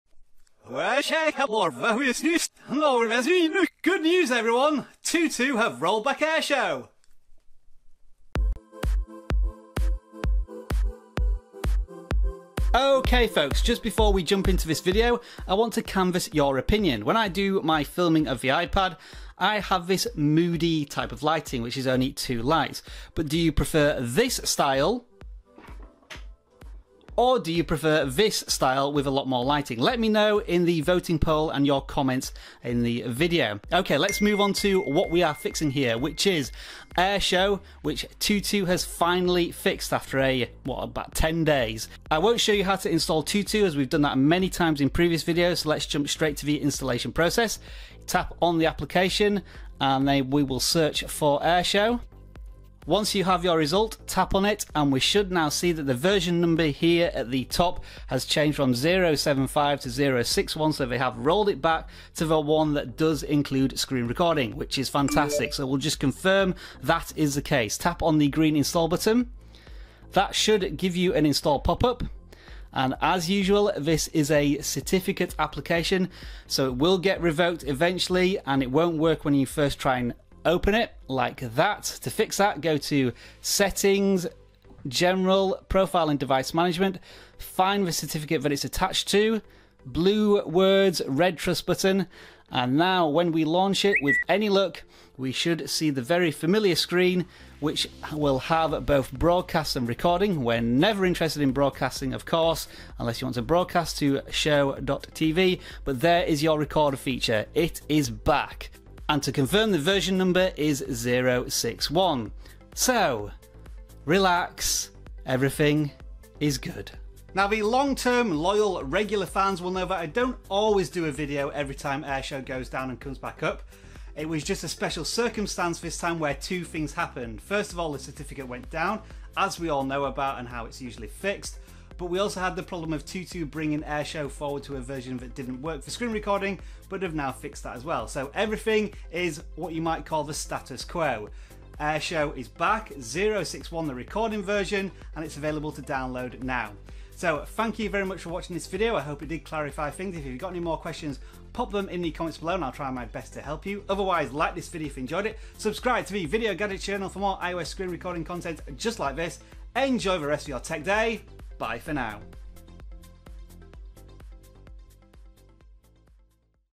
Airshou on Tutu: Is FIXED! How to Record your iPhone and iPad Screen
Good news everyone. Our favourite iOS screen recorder, Airshou, has been rolled back to version 0.6.1. on Tutu! I probably shouldn't have told you that in the video description as you won't watch the video now. I'm an idiot!

Anyway, enjoy and rejoice!